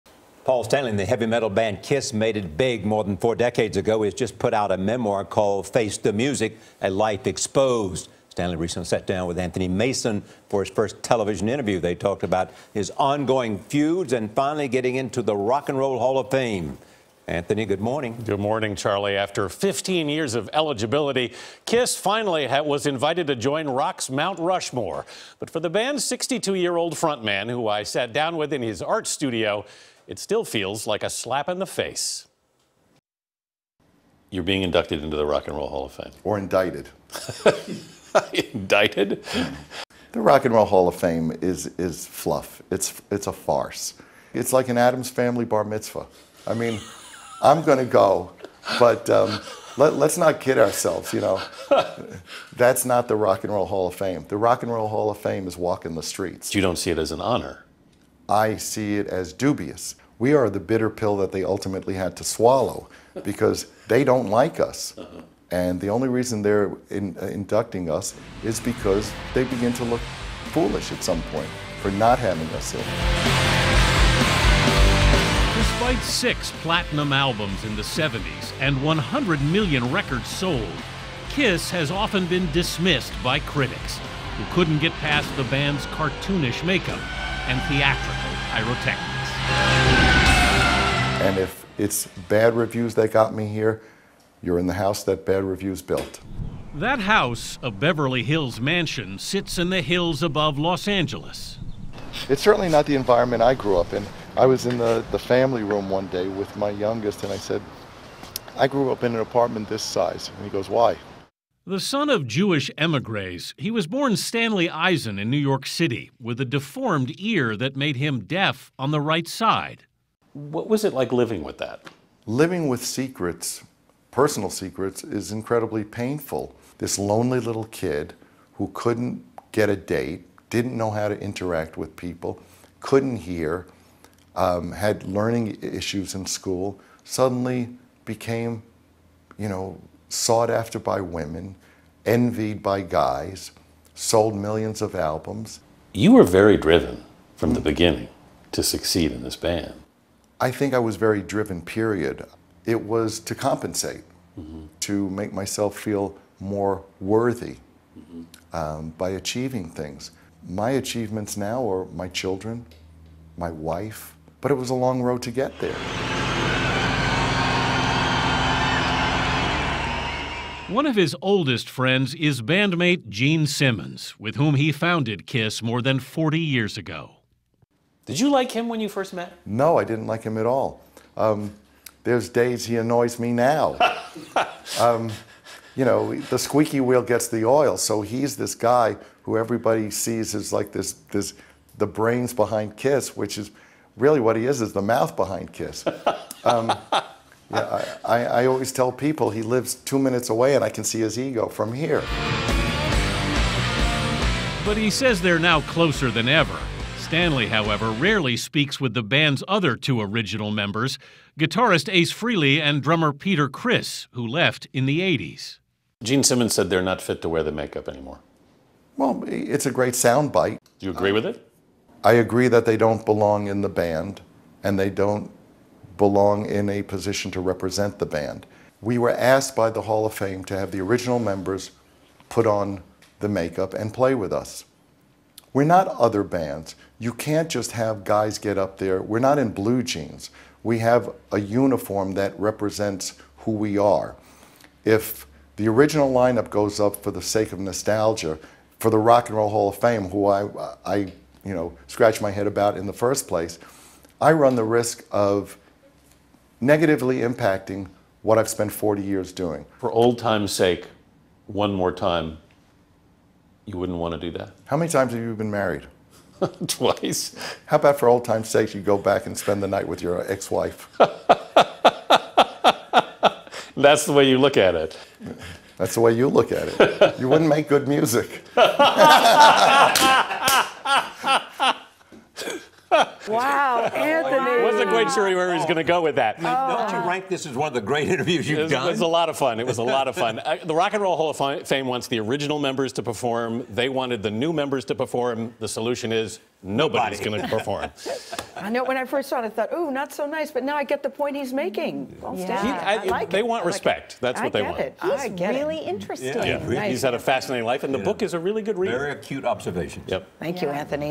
KISS star Paul Stanley on fame, feuds and secrets
KISS front man Paul Stanley has written the memoir "Face the Music: A Life Exposed." Stanley sat down with CBS News' Anthony Mason to discuss his ongoing feuds with his former band mates and being inducted into the Rock and Roll Hall of Fame.